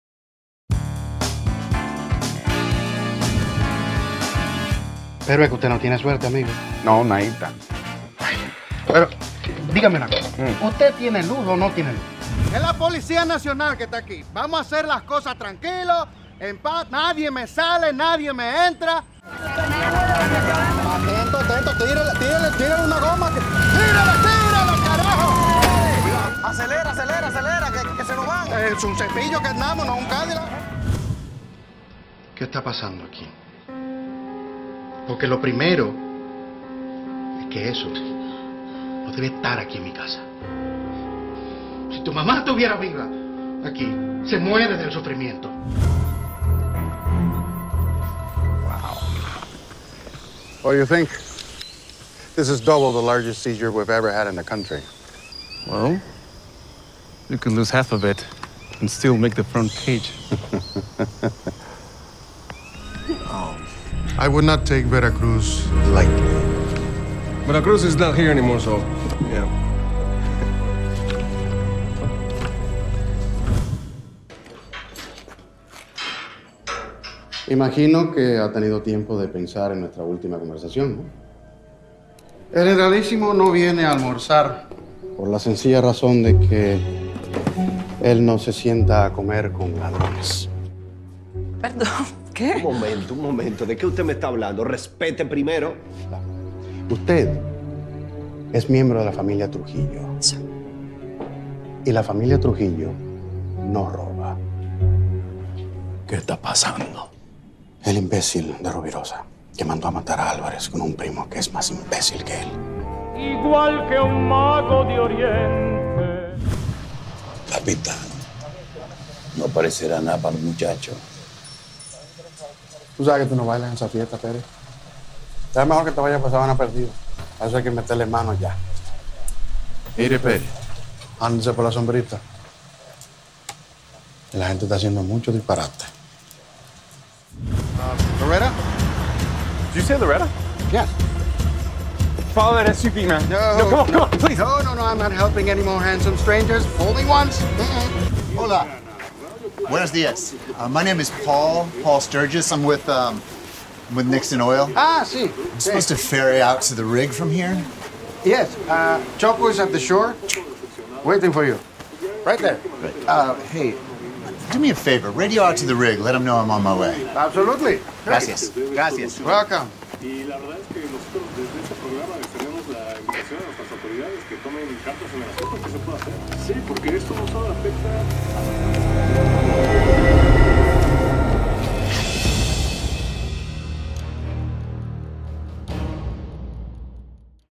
DEMO ACTORAL OMAR PATIN 2024
Este es el demo demo actoral de Omar Patin. Actualizado al año 2024 (Duración 3 Min)